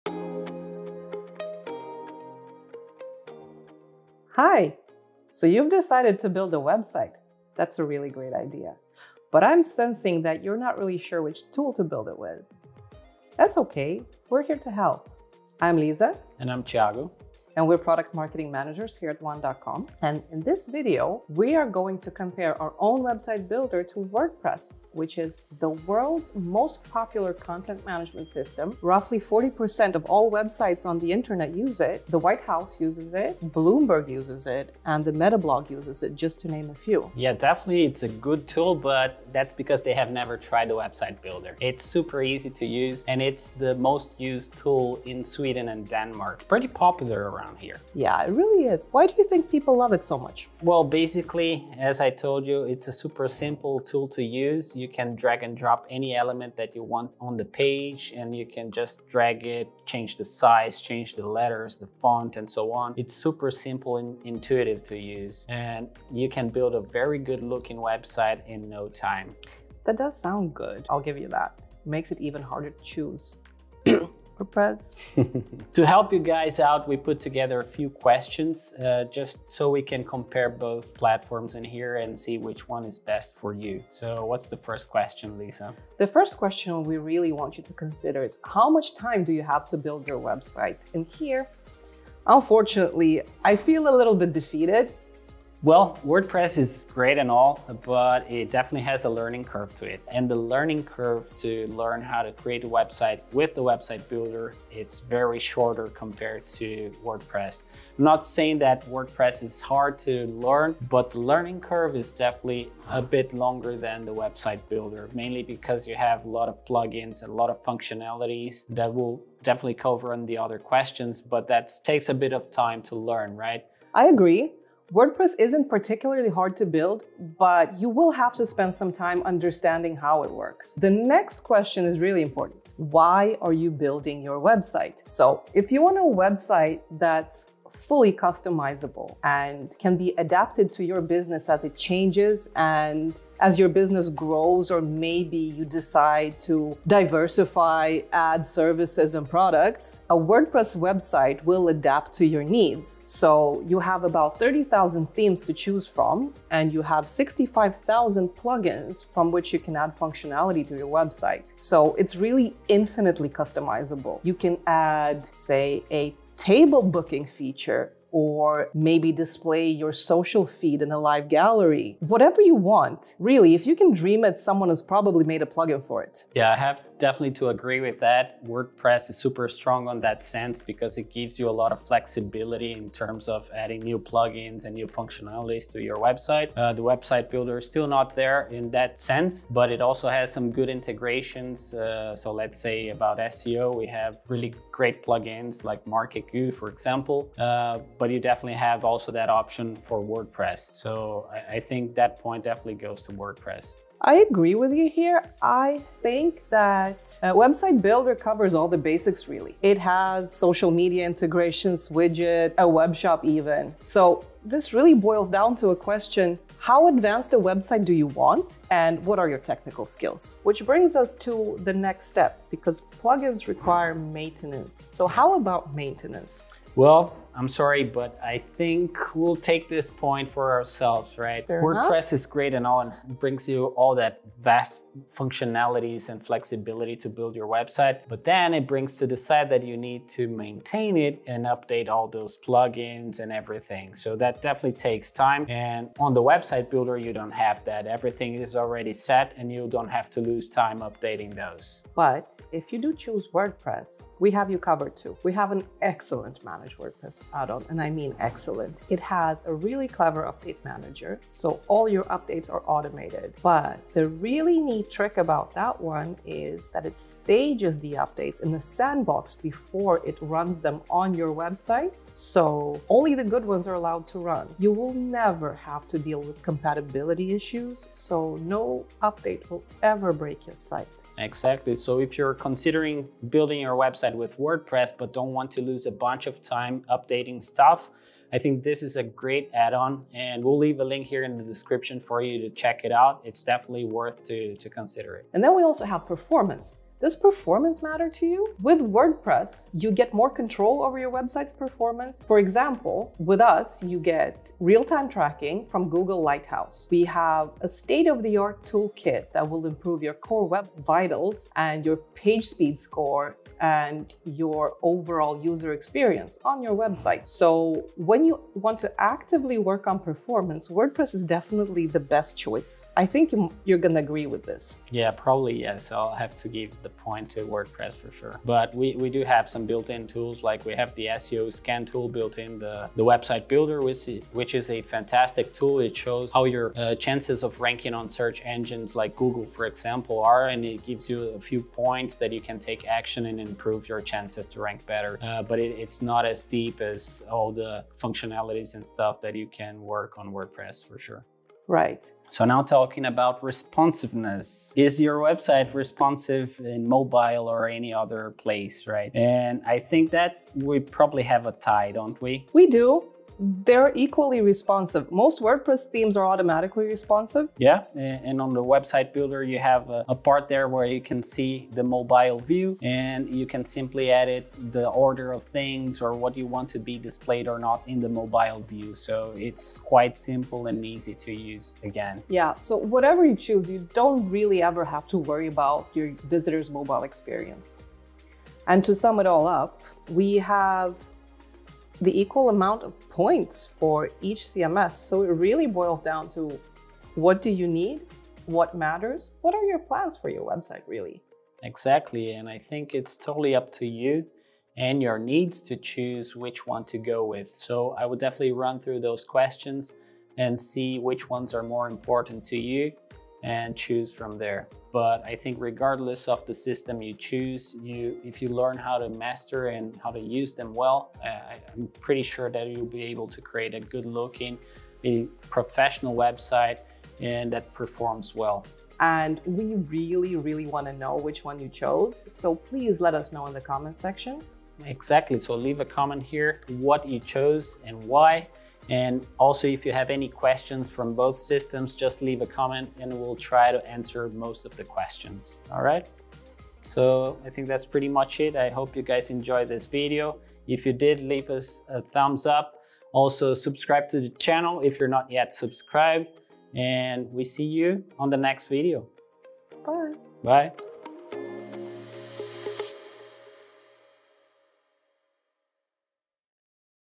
WordPress vs Website Builder: Which tool to use?
One of the most important factors to consider when building your new website is which tool you want to use. Check out this video to find out whether you should go for WordPress or Website Builder.

Timestamps
00:00 - Intro
01:28 - How much time do you have to edit your website?
02:12 - Why are you building your website?
03:43  - Which one is easier to maintain?
04:54 - Which one is better on performance?
05:53 - Which one is more responsive?
06:27 - Conclusion